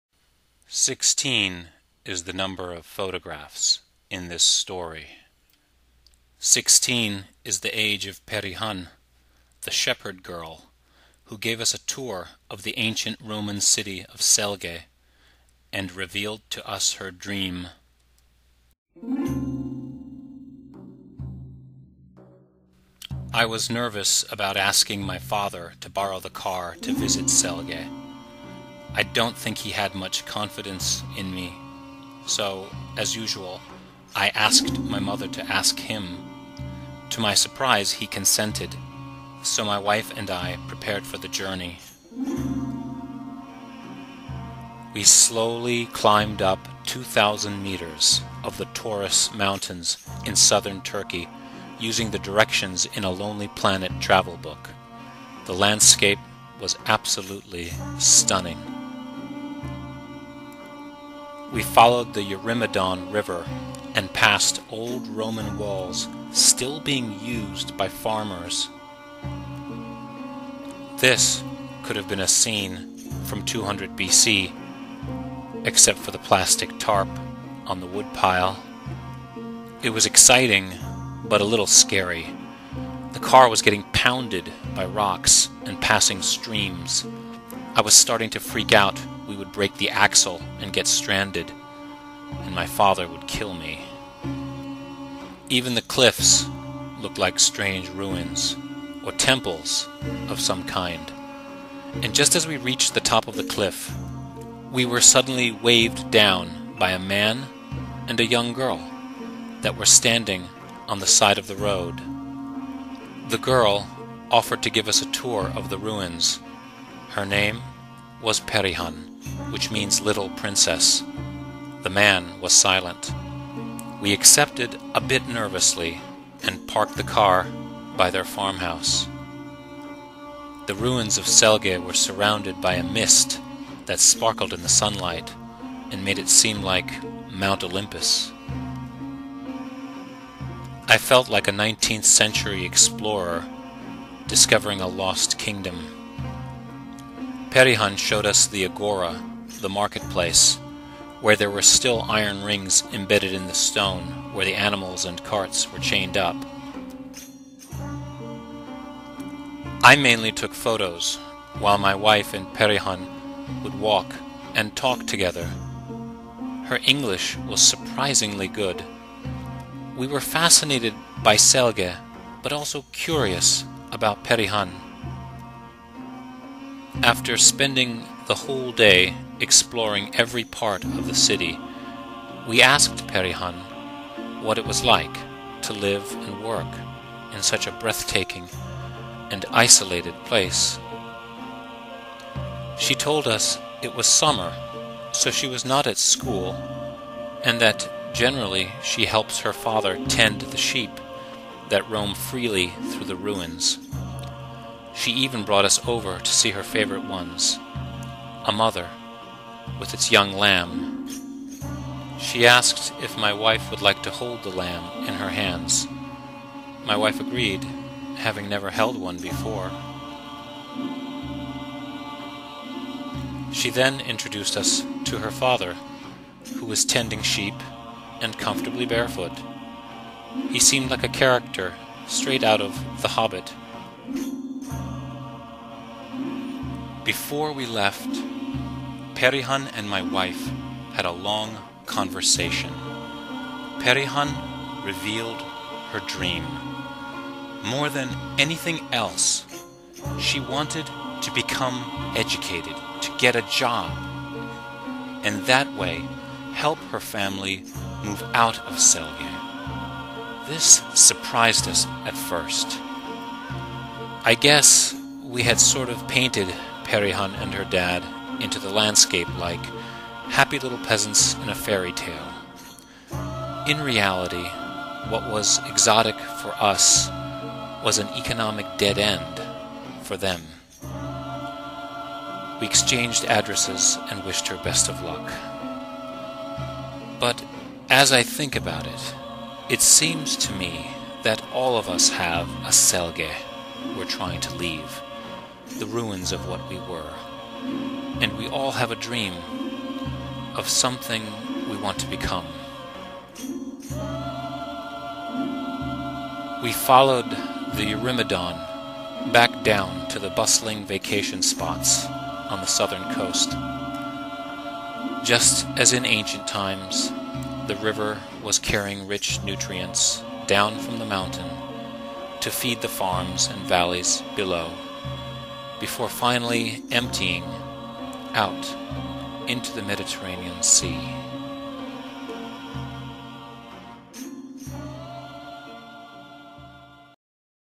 Leaving the Ruins
Digital story by Mustafa Sakarya.  Produced at the Mercy College Faculty Center for Teaching and Learning. The story concerns an encounter with a 16 year old shepherd girl, living and working at Selge, the site of an ancient Roman city, located in the high plateaus of the Taurus Mountains in southern Turkey.